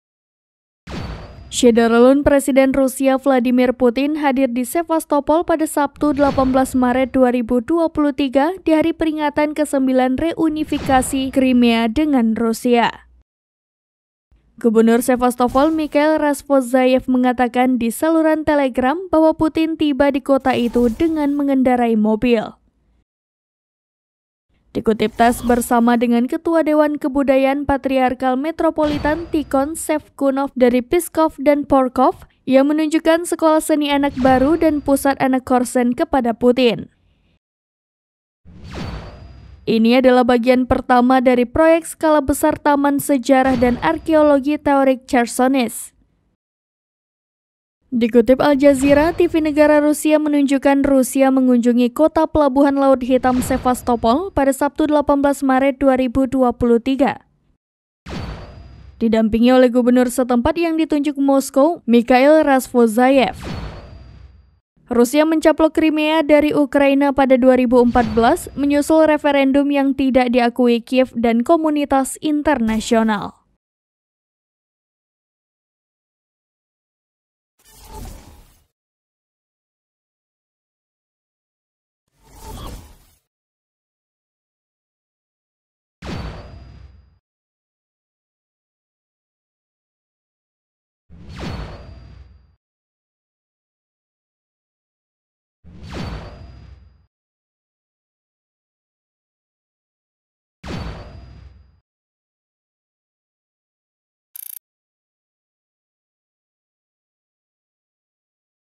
Putin Kemudikan Mobil Sendiri ke Sevastopol, Pejabat Rusia Sebut Pemimpin Luar Biasa
SERAMBINEWS.COM - Presiden Rusia Vladimir Putin hadir di Sevastopol pada Sabtu (18/3/2023) di hari peringatan kesembilan reunifikasi Krimea dengan Rusia.

Gubernur Sevastopol Mikhail Razvozhayev mengatakan di saluran Telegram bahwa Putin tiba di kota itu dengan mengendarai mobil.

Dikutip TASS, bersama dengan ketua Dewan Kebudayaan Patriarkal, Metropolitan Tikhon (Shevkunov) dari Pskov dan Porkhov, ia menunjukkan sekolah seni anak baru dan pusat anak Korsun kepada Putin.

Ini adalah bagian pertama dari proyek skala besar taman sejarah dan arkeologi Tauric Chersonese.

Dikutip Al Jazeera, TV negara Rusia menunjukkan Rusia mengunjungi kota pelabuhan Laut Hitam Sevastopol pada Sabtu (18/3/2023), didampingi oleh gubernur setempat yang ditunjuk Moskow, Mikhail Razvozhayev.

Rusia mencaplok Krimea dari Ukraina pada 2014 menyusul referendum yang tidak diakui Kyiv dan komunitas internasional.(*)

VE: Teuku Raja Maulana